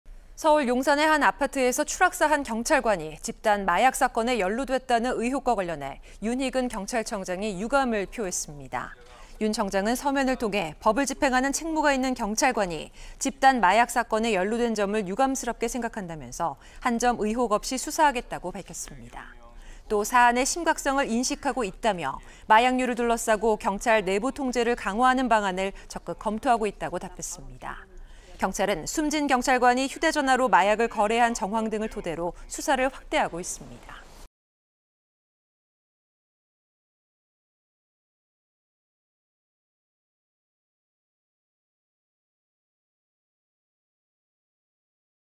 경찰청장 "추락사 경찰관 집단 마약 연루 유감" / OBS 뉴스O
서울 용산의 한 아파트에서
추락사한 경찰관이
집단 마약 사건에 연루됐다는
의혹과 관련해 윤희근 경찰청장이
유감을 표했습니다.

윤 청장은 서면을 통해 법을 집행하는 책무가 있는 경찰관이 집단 마약 사건에 연루된 점을 유감스럽게 생각한다면서 한 점 의혹 없이
수사하겠다고 밝혔습니다.

또 사안의 심각성을 인식하고 있다며 마약류를 둘러싸고 경찰 내부 통제를 강화하는 방안을
적극 검토하고 있다고 답했습니다.

경찰은 숨진 경찰관이 휴대전화로 마약을
거래한 정황 등을 토대로 수사를 확대하고 있습니다.